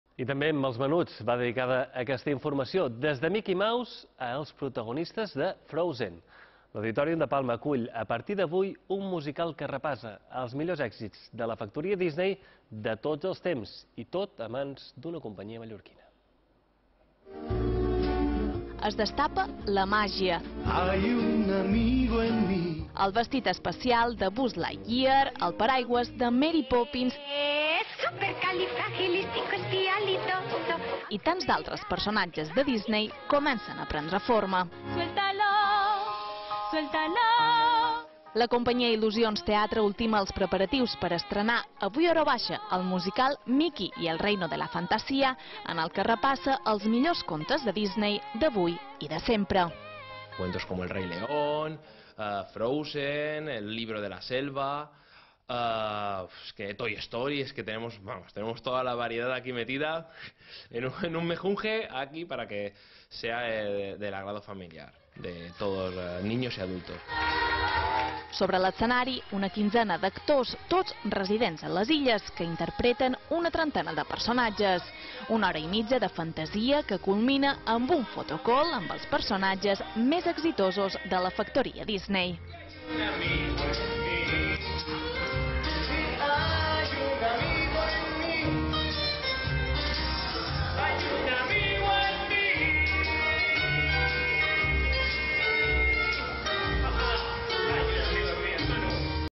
La música de Disney d’avui i de sempre, a l’Auditòrium
Des de Mickey Mouse... a les protagonistes de Frozen. L'Auditorium de Palma acull a partir d'avui un musical que repassa els millors èxits de la factoria Disney, i tot a mans d'una companyia mallorquina. Sobre l'escenari, una quinzena d'actors, que interpreten una trentena de personatges. Una hora i mitja de "Mickey y el Reino de la Fantasía", que culmina amb un photocall amb els personatges més exitosos de Disney.